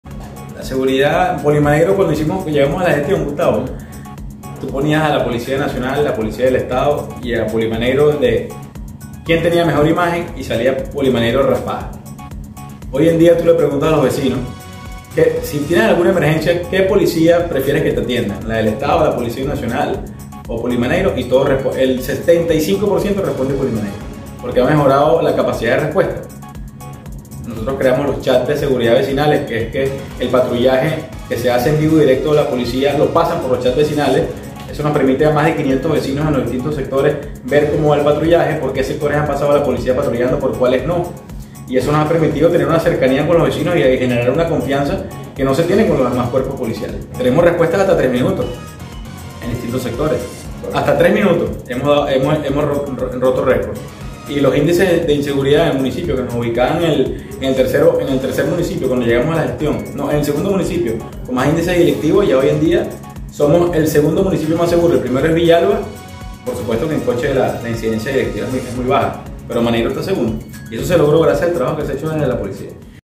De ser el 2do municipio con mayor delincuencia pasamos a ser el 2do más seguro del Estado
Hace 4 años, cuando iniciamos nuestra gestión, el solo nombrar "polimaneiro" la gente se asustaba,  su imagen se encontraba en el piso. Tuvimos que hacer una limpieza general, de los 100 funcionarios que conformaban la policía, quedaron solo 44, hoy ya son 150 con un parque automotor presto, así como las motos y el cuerpo ciclista, con un tiempo de respuesta de 3 minutos.